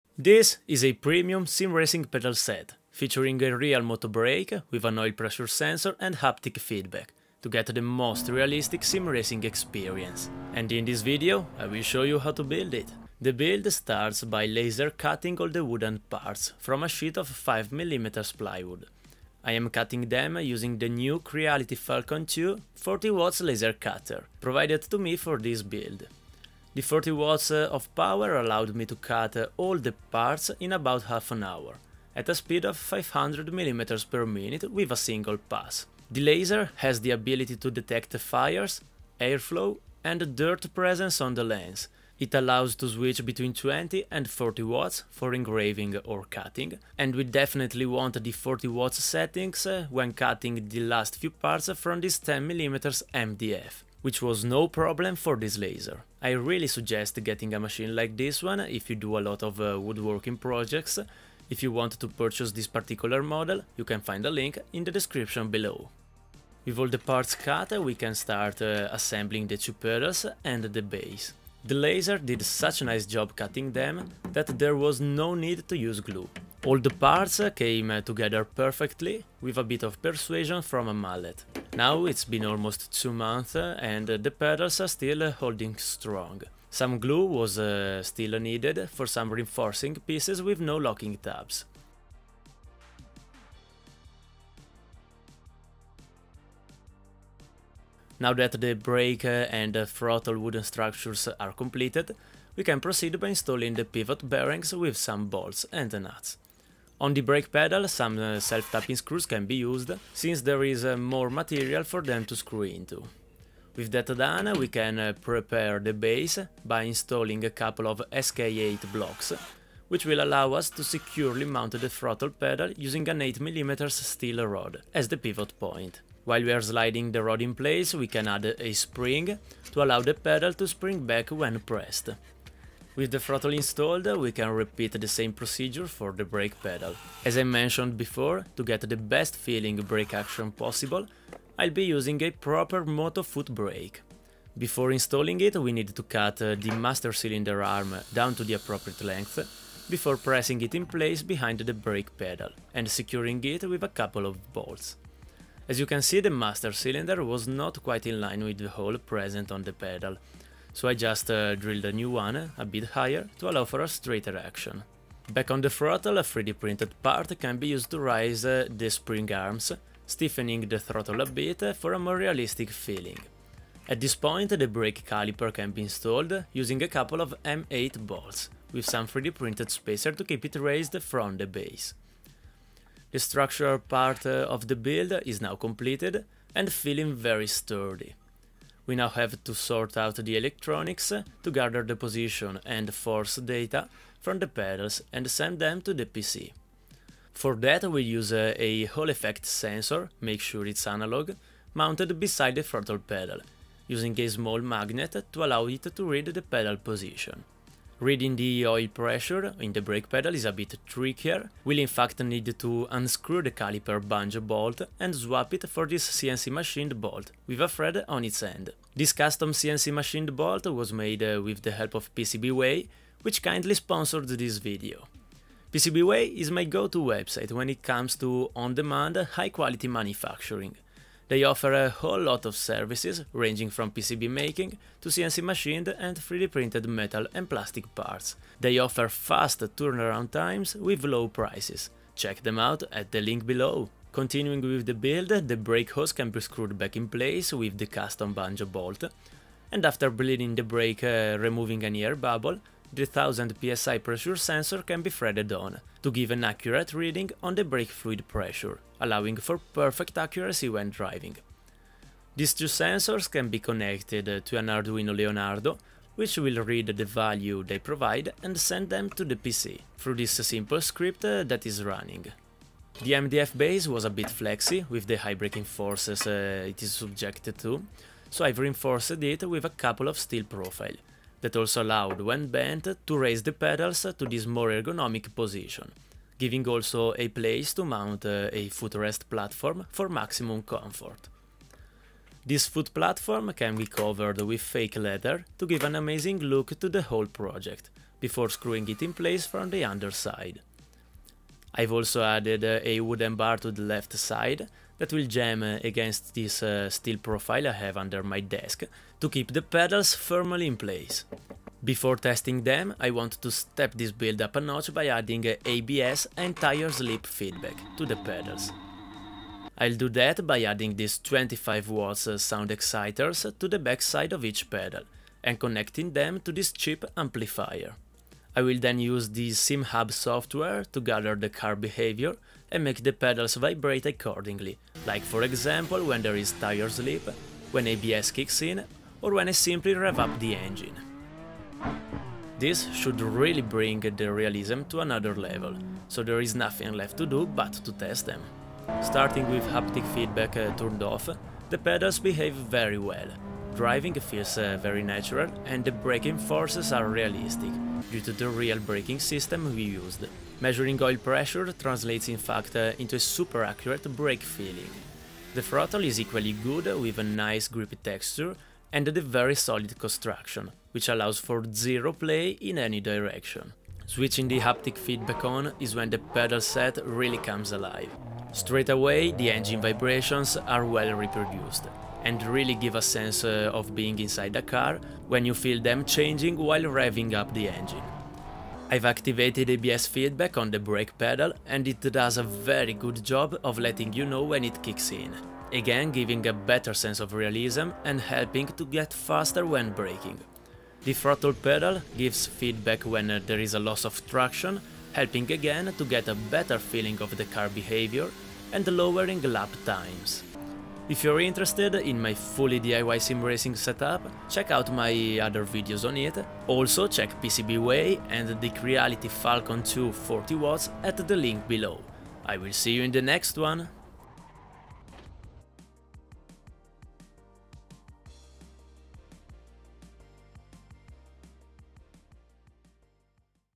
DIY sim racing PEDALS, 150$, super realistic
Code: FN1106B
Code: FN1107EE

in this video I'll show you how to build a high-performance pedal set for a fully DIY racing simulator. I designed it to be simple to build, with few moving pieces, and to be cut out of standard hardware store plywood.